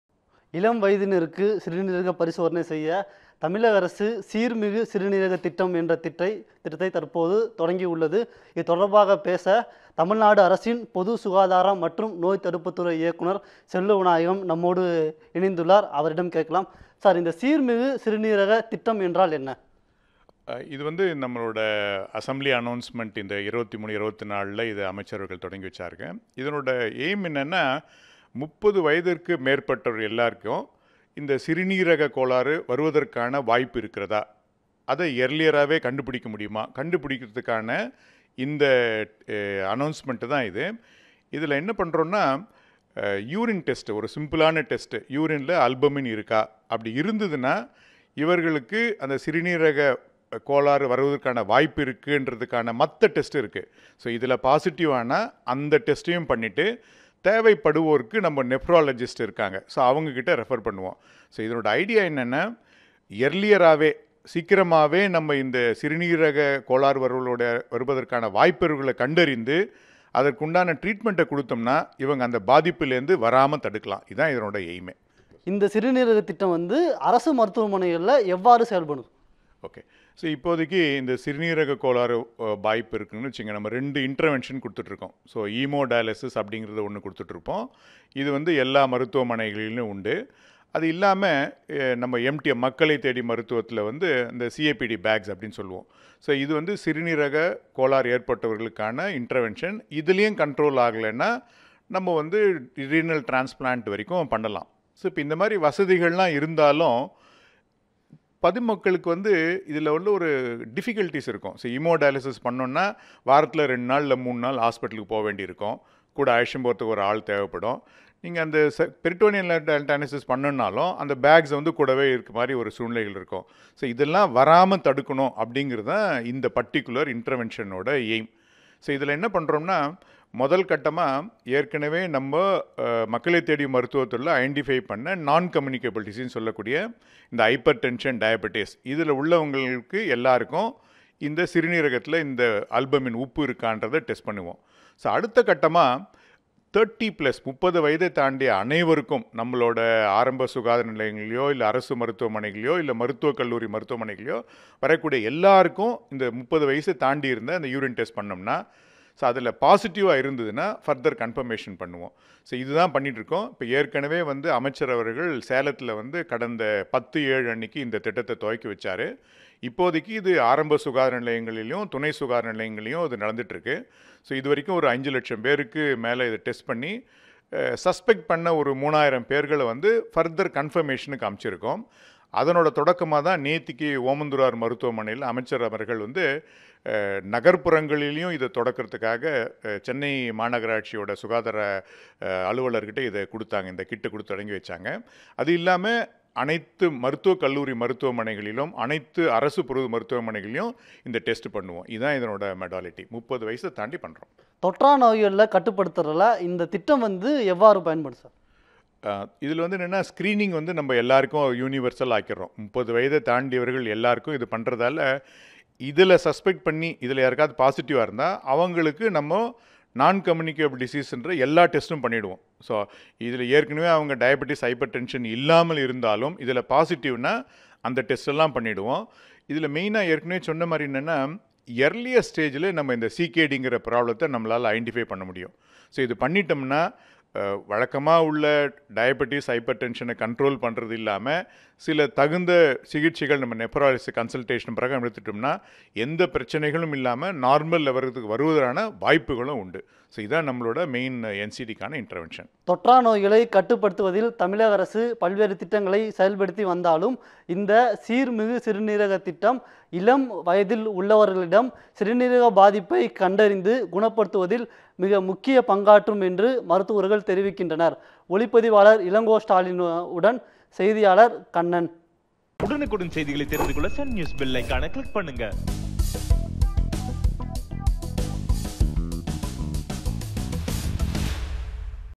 ‘சிறுநீரகம் காக்கும் சீர்மிகு மருத்துவ திட்டம்' யாரெல்லாம் பயன்பெறலாம்!!! | Sun News
Credits : @Sunnewstamil

Dr. T. S. Selvavinayagam, MD, DPH, DNB, - Director of Public Health and Preventive Medicine